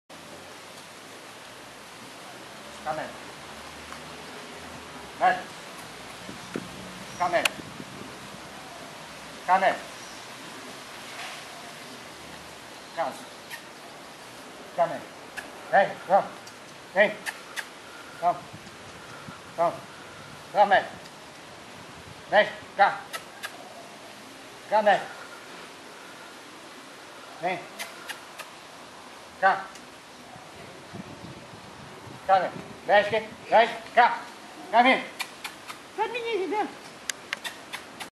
VÍDEO DO WINDHAVEN MAGIC TOUCH AOS 8 MESES
ESTE É O WINDHAVEN MAGIC TOUCH AOS 8 MESES. PRESENTE VALIOSO DO NOSSO AMIGO, CRIADOR E ÁRBITRO ANTONIO DA COSTA DALLE PIAGGE, PROPRIETÁRIO DO CANIL WINDHAVEN COLLIES/SP